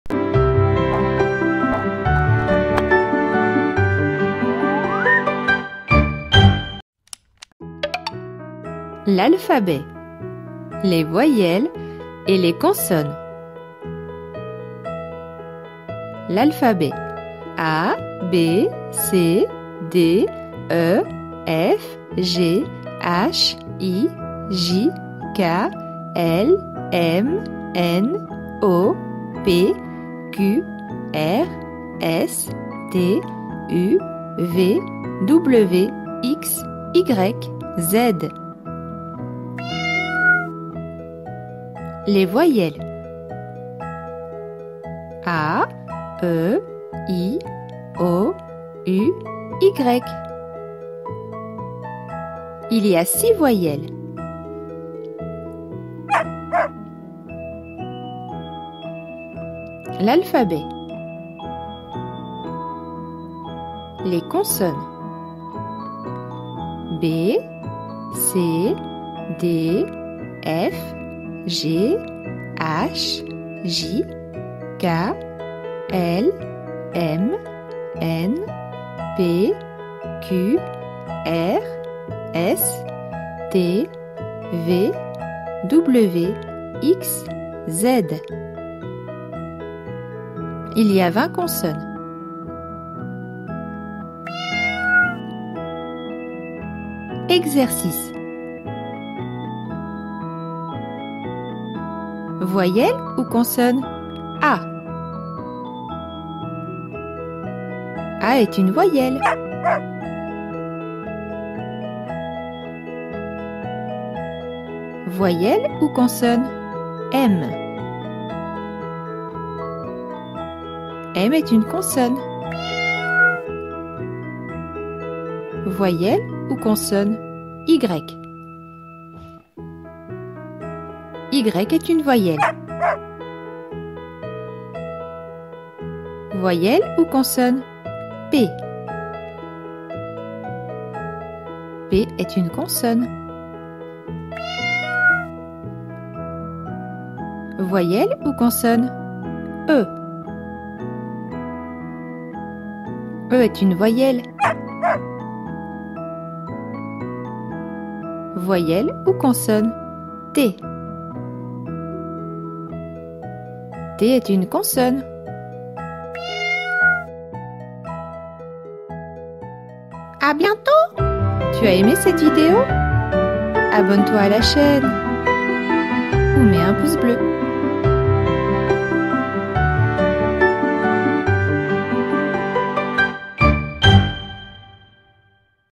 L'Alphabet : les Voyelles et les Consonnes (nouvelle version)
Apprenez l'alphabet, les voyelles et les consonnes avec petit chat et petit chien. La leçon est suivie de plusieurs exercices.